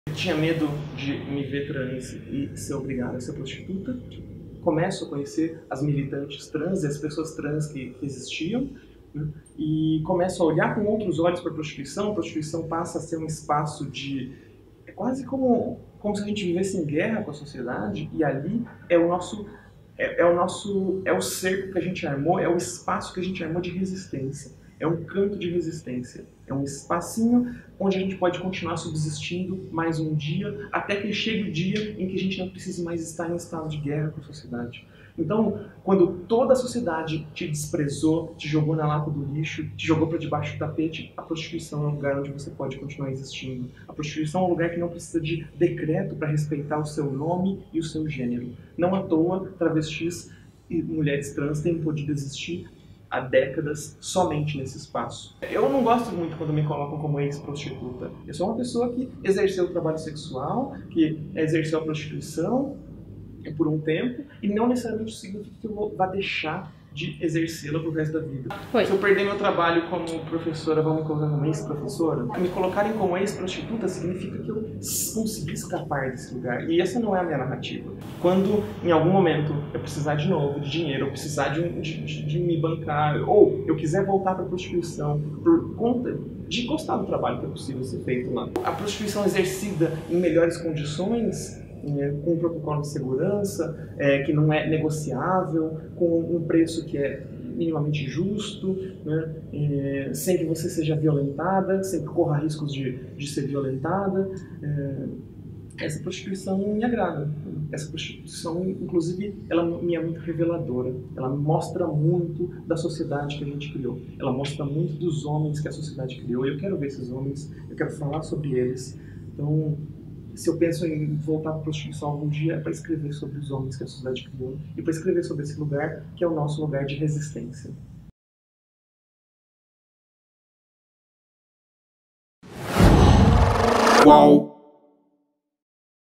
AMARA MOIRA: "NÃO GOSTO QUANDO ME COLOCAM COMO EX-PROSTITUTA"
Amara Moira, 32, se apresenta como travesti, feminista e escritora. Referência nos debates de gênero, com mais espaço no Brasil a cada dia, é dona de uma voz delicada e uma sensibilidade sem igual para explicar as causas pelas quais luta.

Bem-vindo ao Canal do UOL Estilo.